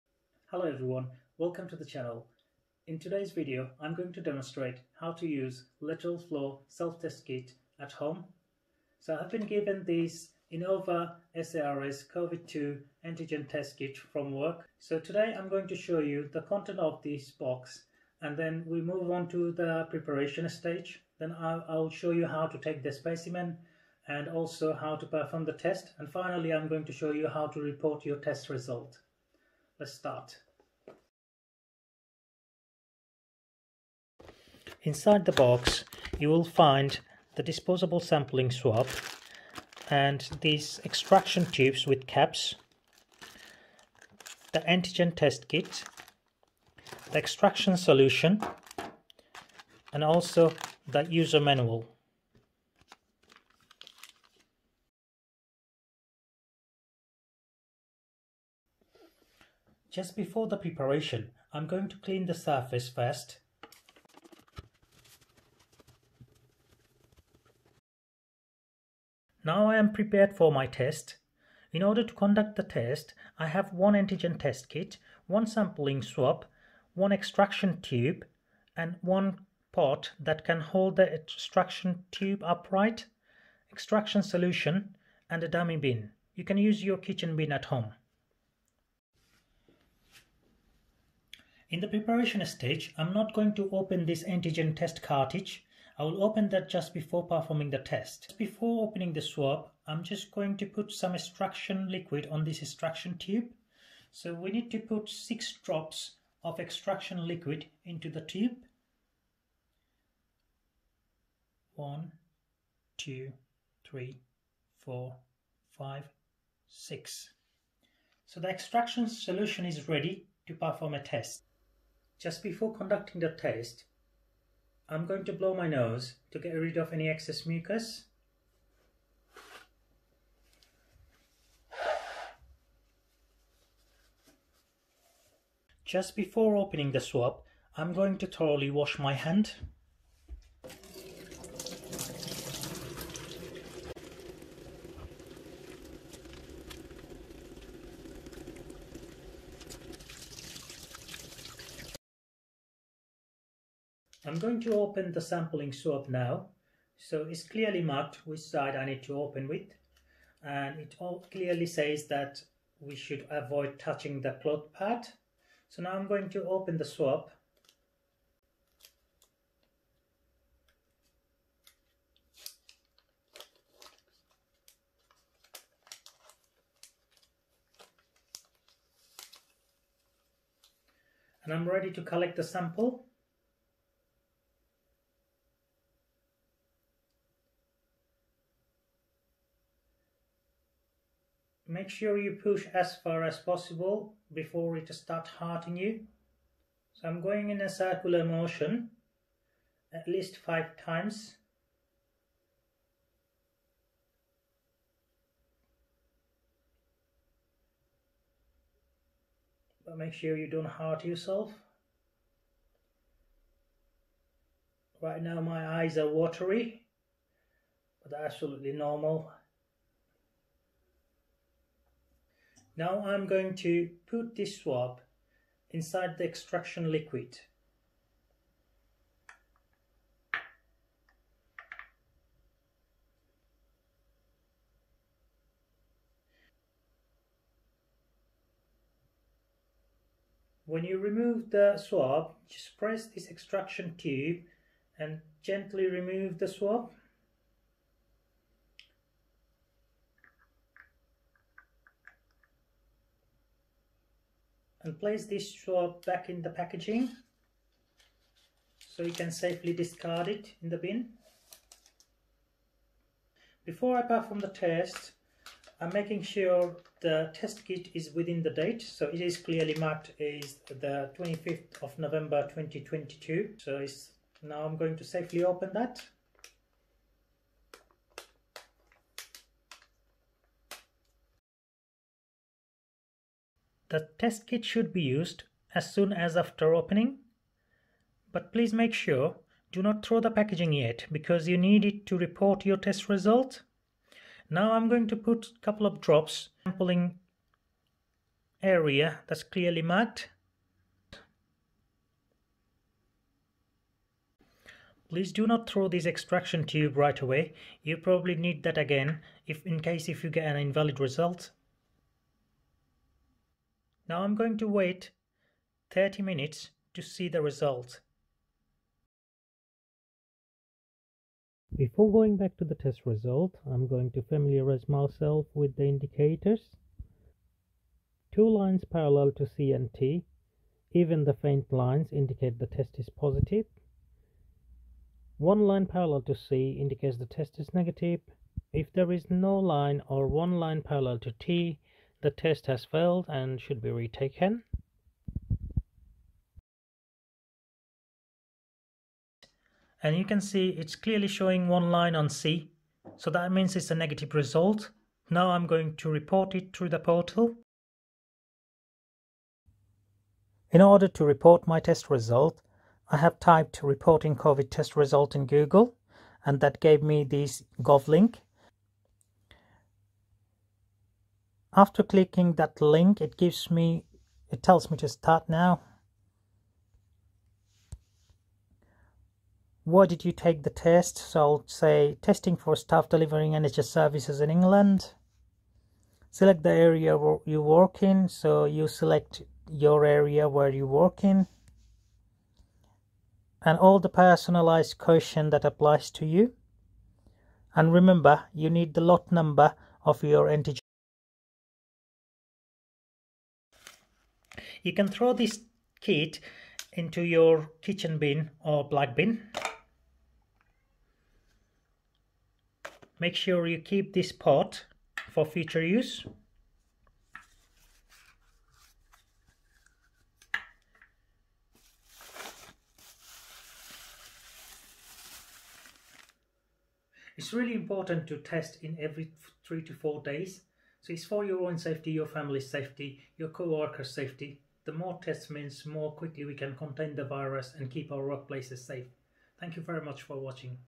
Covid-19 home test by using Innova SARS Cov-2 Antigen rapid qualitative test. Lateral flow self test
Covid-19 home test by using Innova SARS Cov-2 Antigen rapid qualitative test. Lateral flow self test device. How to report self Covid-19 testing.